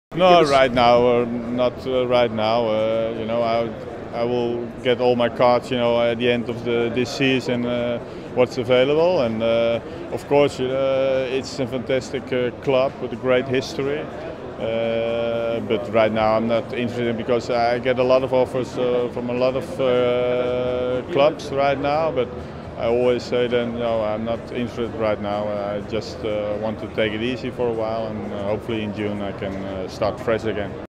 De Boer rules himself out of Rangers job
Former Rangers defender Frank de Boer has ruled himself out of the running to replace Mark Warburton at Ibrox, insisting he is "taking a break from management".